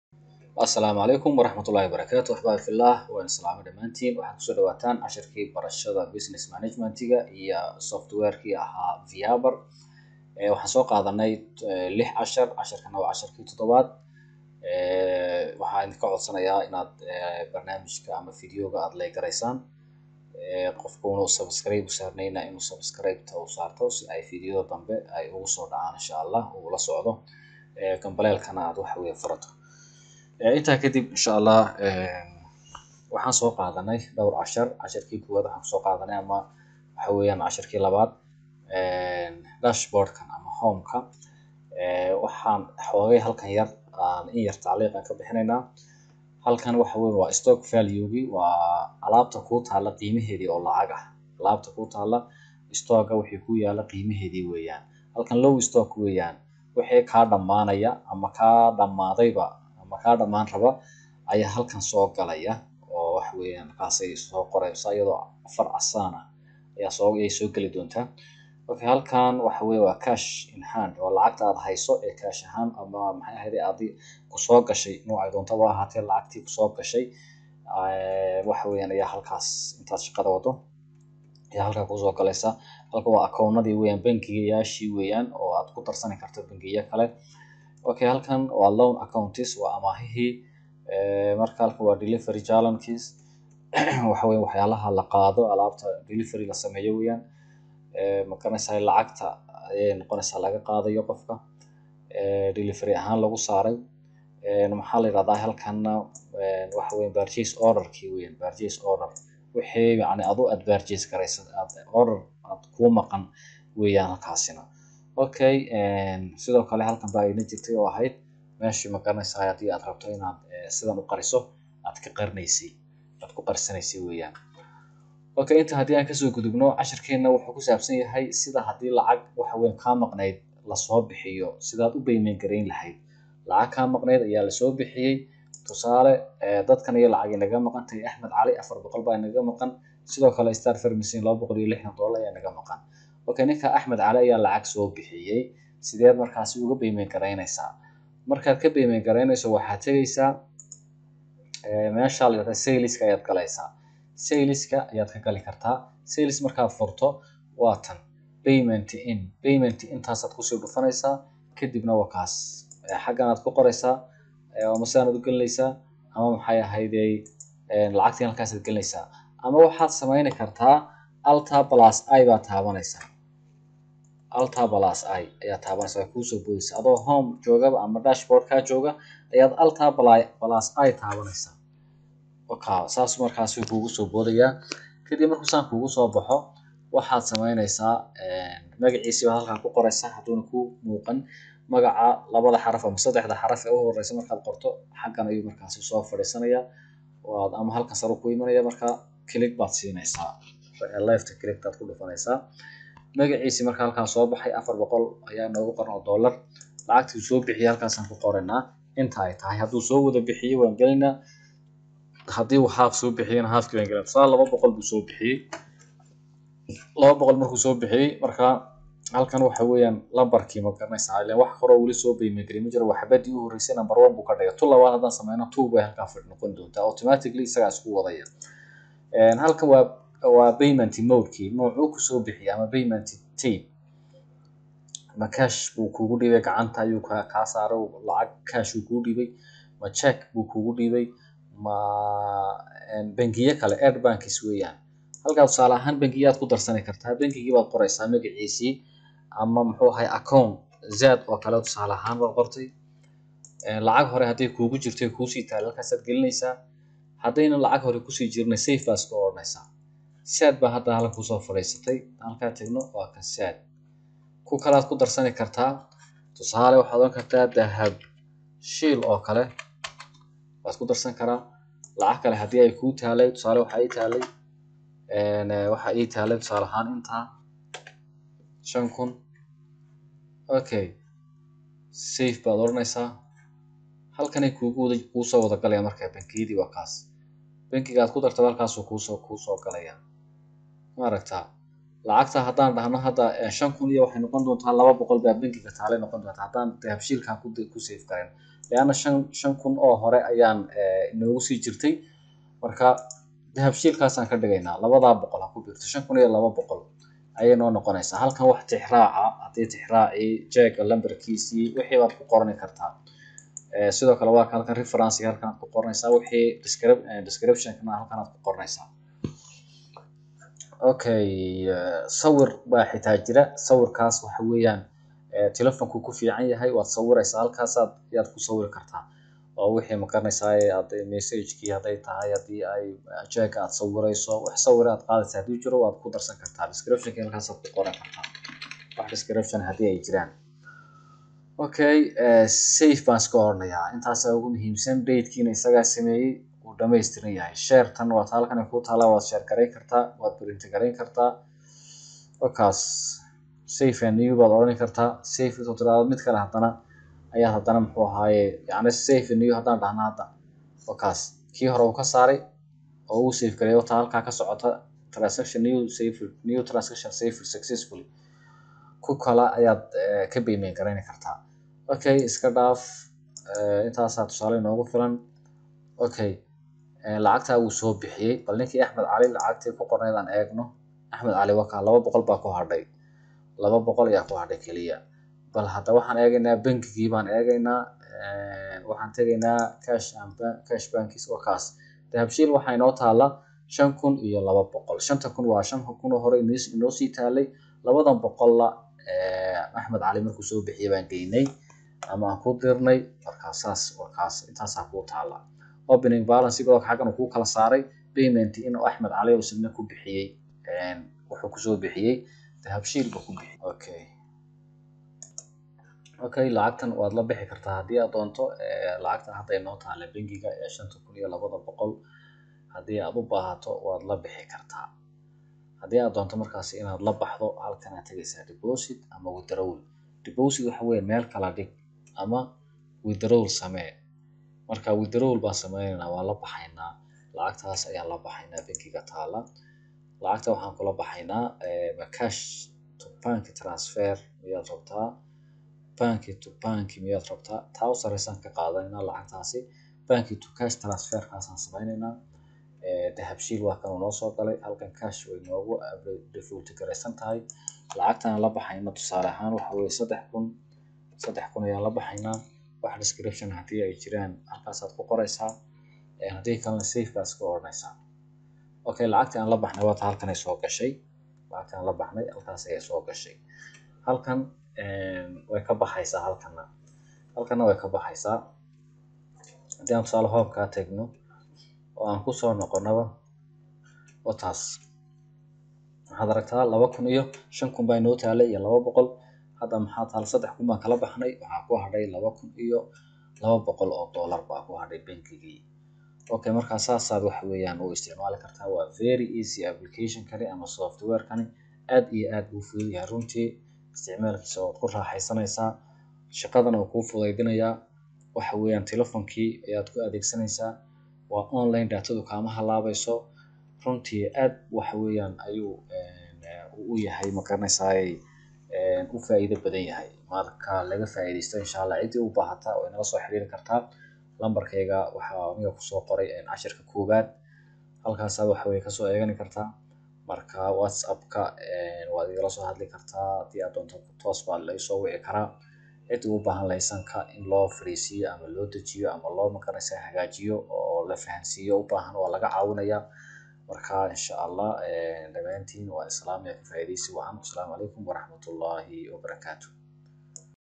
Casharkii 7aad Vyapar Sida loo sameyo Payment ama loga saaro lacag la so bixiyay how to make payment
Casharkii 7aad Vyapar Sida loo sameeyo Payment ama looga saaro lacag la soo bixiyay